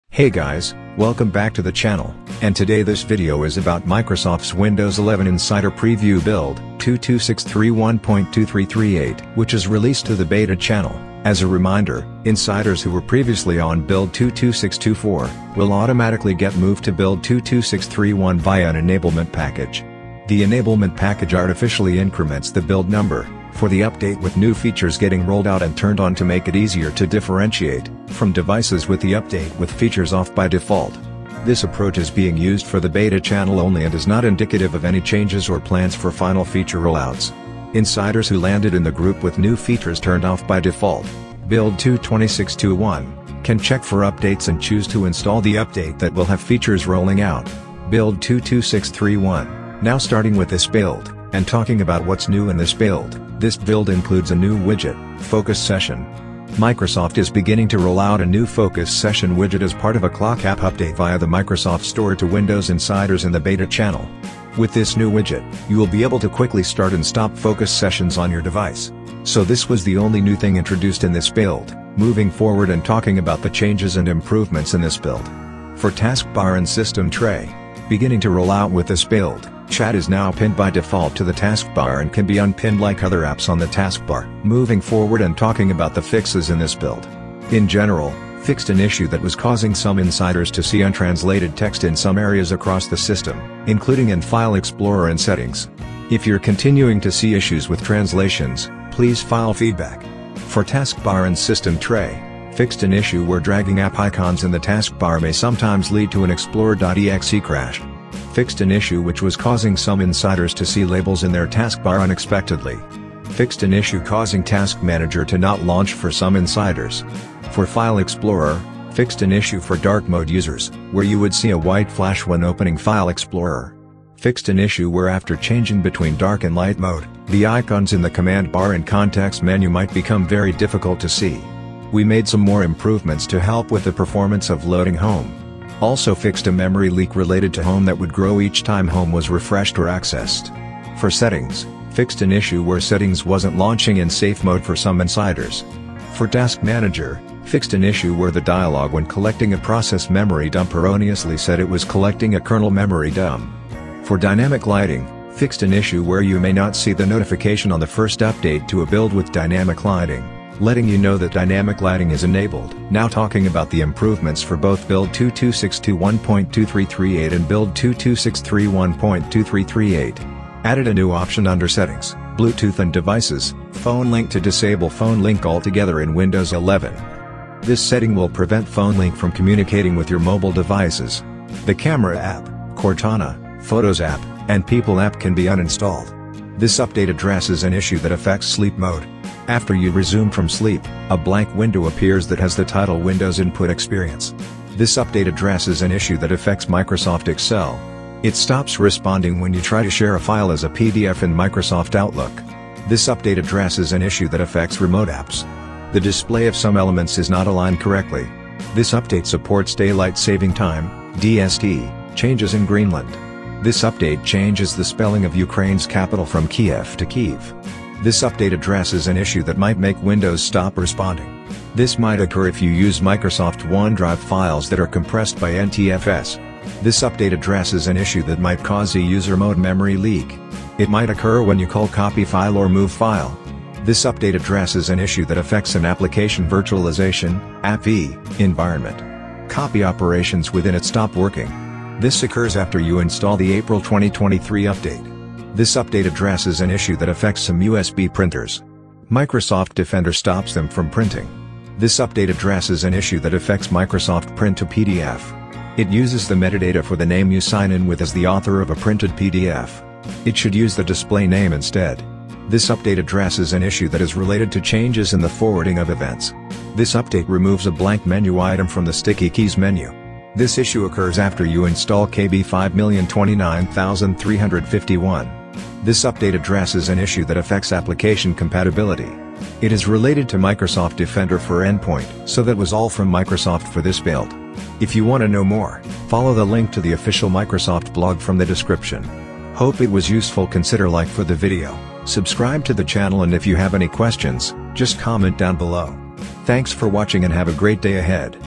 Windows 11 Insider Preview Build 22631.2338: New Widget, Changes, Improvements, Fixes & More
CHAPTERS

GET SUPPORT
► Support:
► Forum: